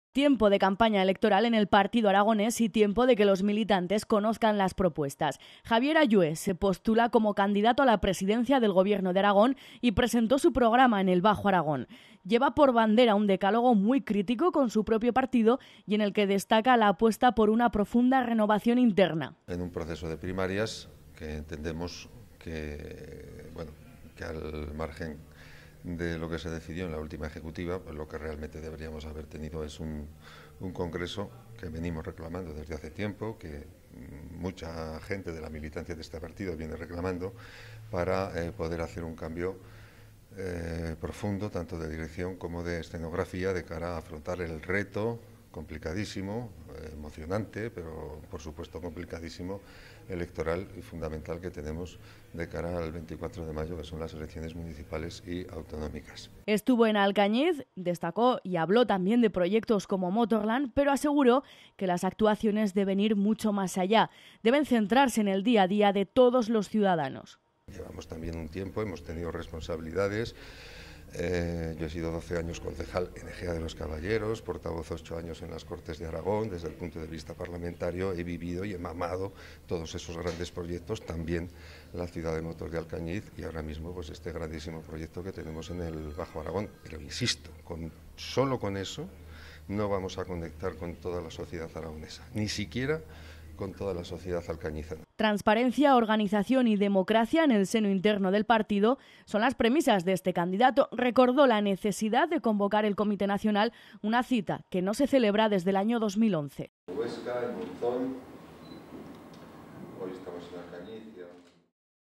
La Comarca.tv - Javier Allué, Primarias PAR
El candidato a las primarias del PAR para el Gobierno de Aragón, Javier Allué, está convencido de que el partido necesita un cambio "radical" para conectar con la sociedad aragonesa